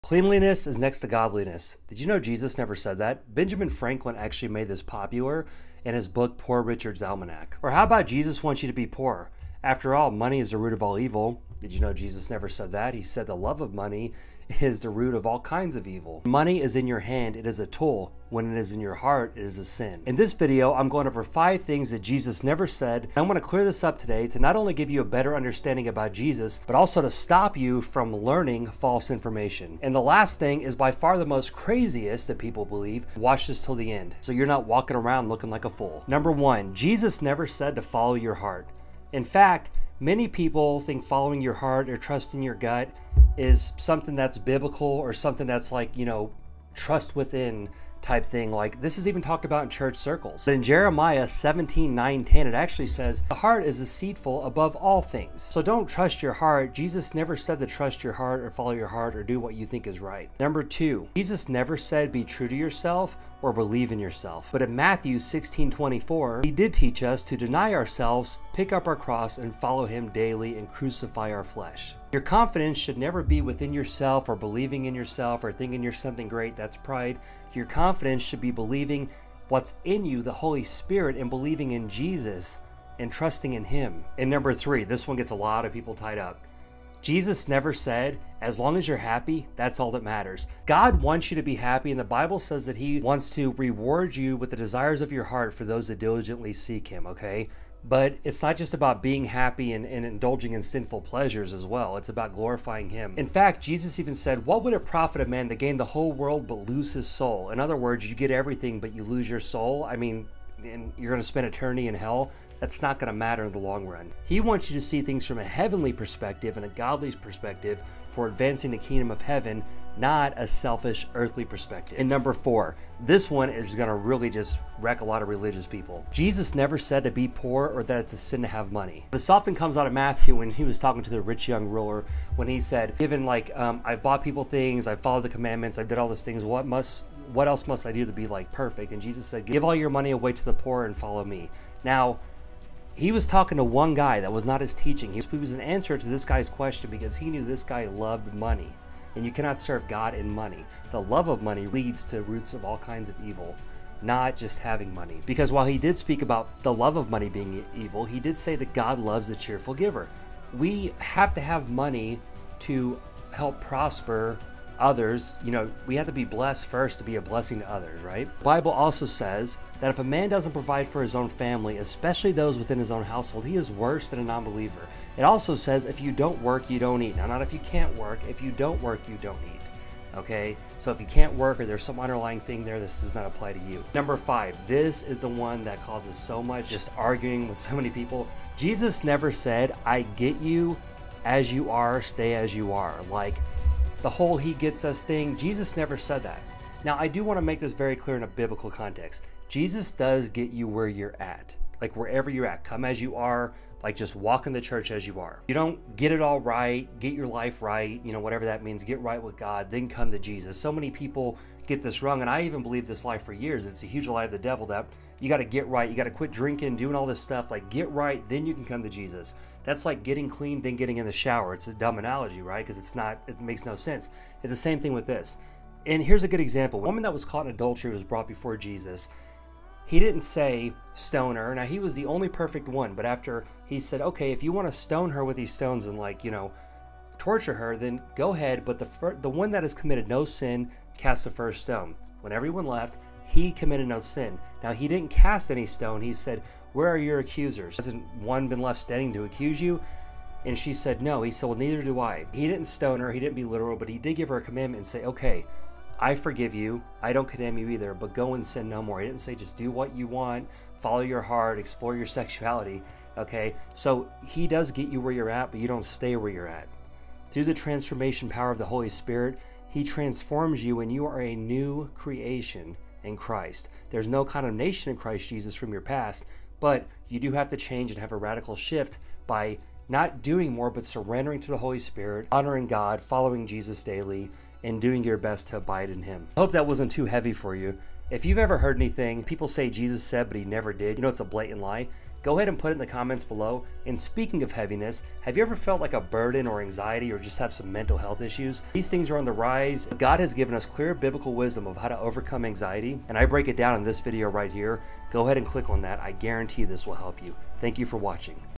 99% Of Christians Get These Wrong
In this video i break down five of the most common lies christians believe. These are repeated around the church and instead of doing a bible study christians just keep saying them and falling into the Devil's trap!